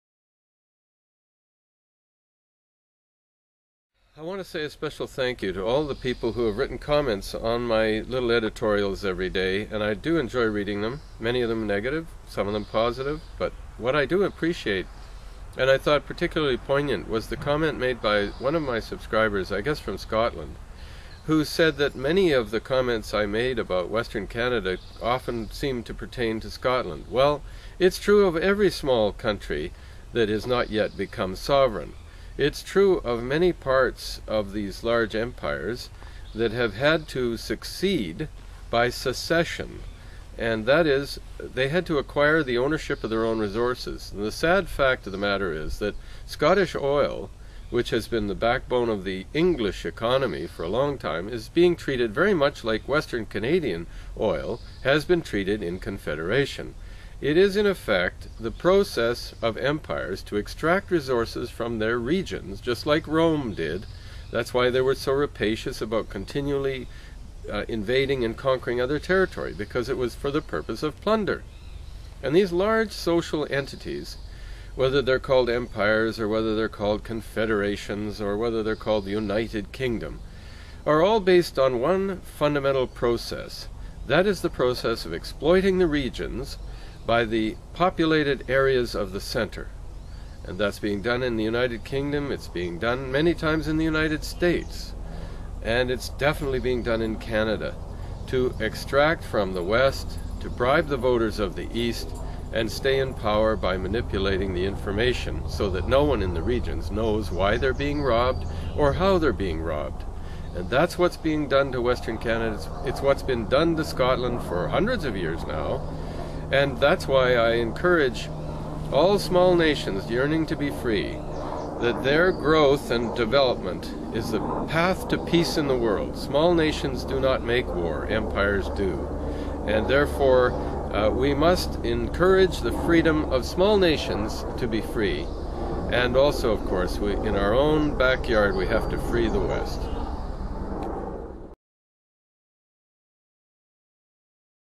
Small Nations Yearning to be Free
Western Separatist Douglas Christie speaks on the value of small nations, in his reply to comments about Scotland and Western Canada. Another reason to free the west.